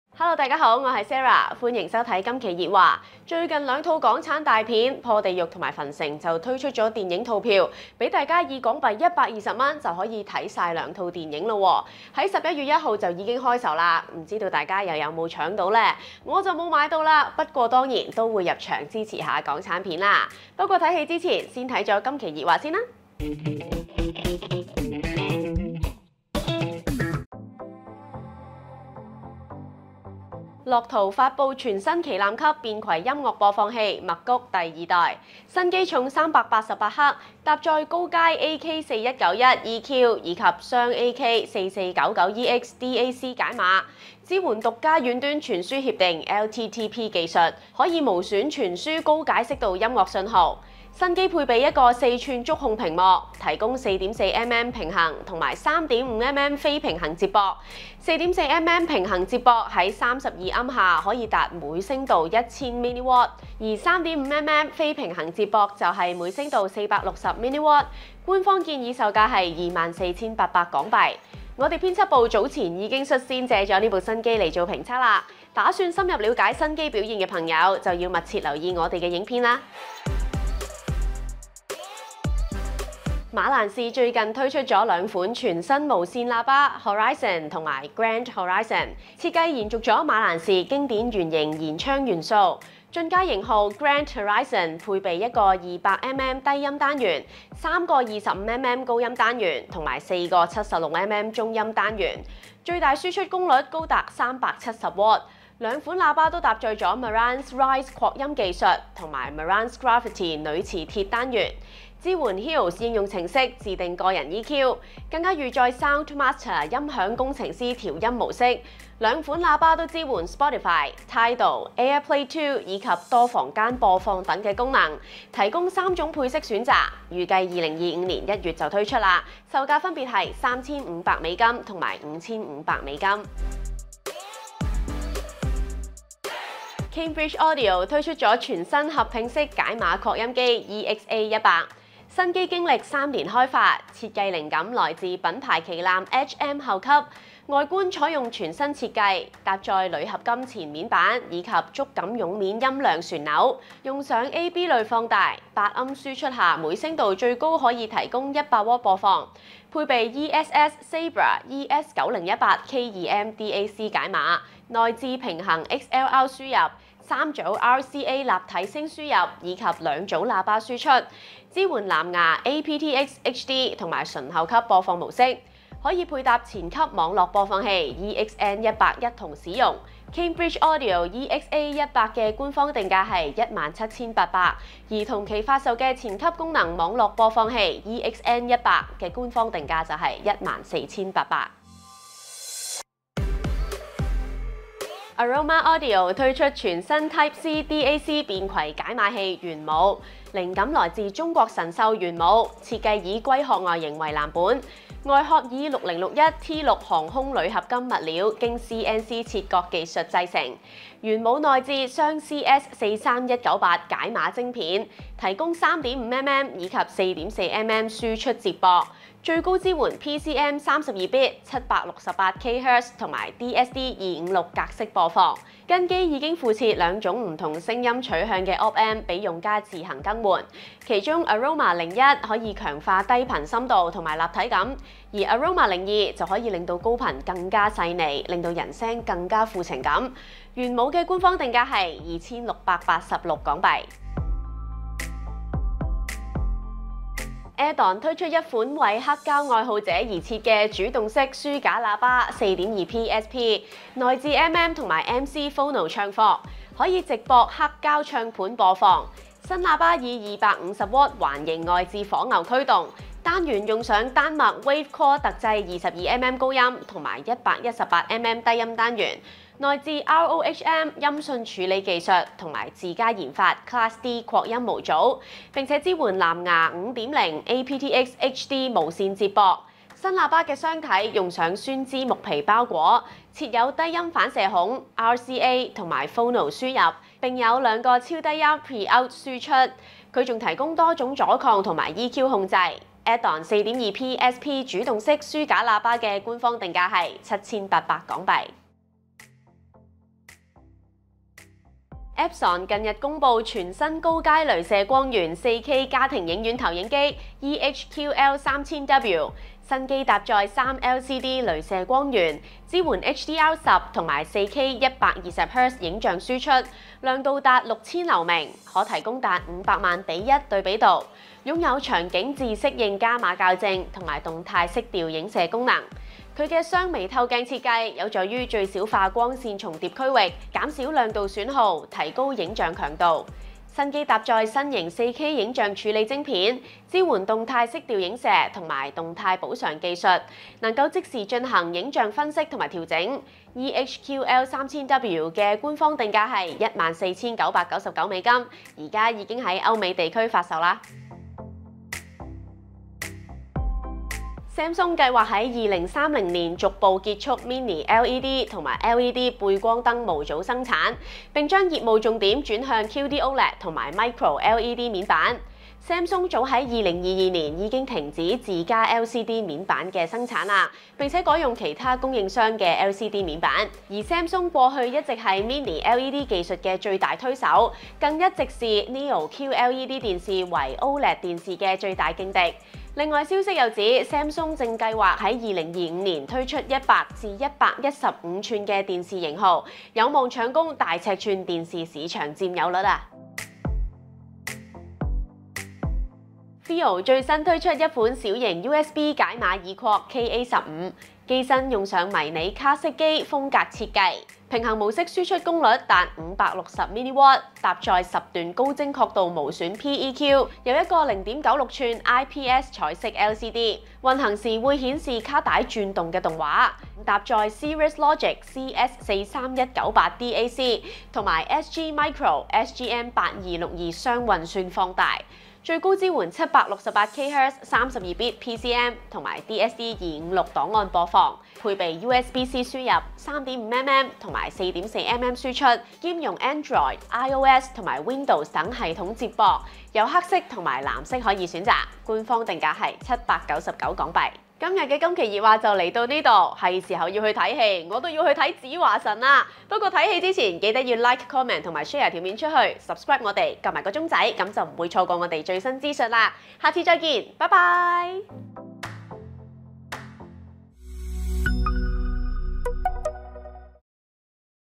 🙋‍♀️今期熱話 : Lotoo PAW GT2 旗艦級音樂播放器 | Cambridge Audio EXA100 合併式解碼擴音機 | AROMA「玄武」全新便攜解碼耳擴
💥今期熱話本集內容!!
💃00:00 Sarah同大家傾下計
💃00:28 Lotoo PAW GT2 旗艦級便攜音樂播放器
💃01:20 Marantz 全新無線喇叭 Horizon 和 Grand Horizon：主打華貴簡約風格
💃02:14 Cambridge Audio EXA100 合併式解碼擴音機：進階音響玩家升級之選
💃03:12 AROMA Audio 推出全新 Type-C 便攜解碼 DAC「玄武」
💃04:04 ADON 4.2P SP 主動式書架喇叭:專門針對黑膠玩家而設
💃04:59 Epson EH-QL3000W 高階鐳射光源 4K 投影機：支援 4K/120Hz 及低延遲顯示
💃05:51 Samsung 計劃於 2030 年逐步結束 Mini LED 及 LED 背光燈模組生產業務
💃06:33 Fiio 最新推出了小型 USB 解碼耳擴 KA15：大玩卡式帶懷舊風

✨今集主持 : 🇭🇰些華 | Sarah 李蒨怡 @sarah_leesy
✨每星期今期熱話截稿日為星期二上午12:00，如各位同業好友欲刊登資訊請盡早提供新品資料